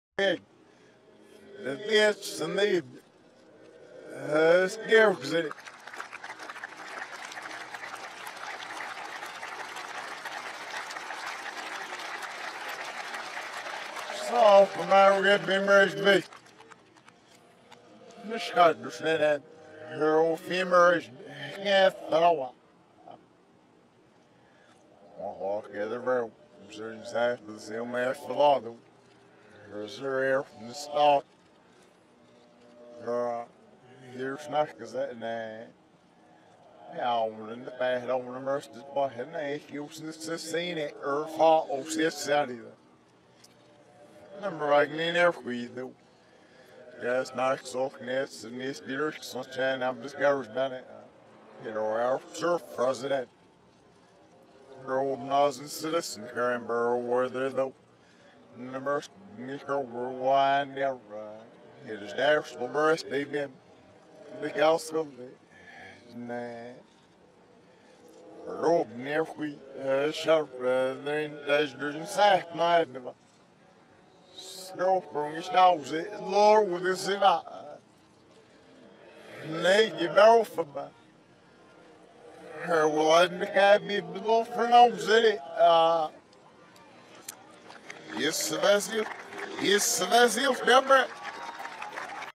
Reverse Speech: Barry Obama's Speech Regarding MH17
Barry gets clearer in reverse every time I listen to him.  I will be updating annotations as I get the chance to.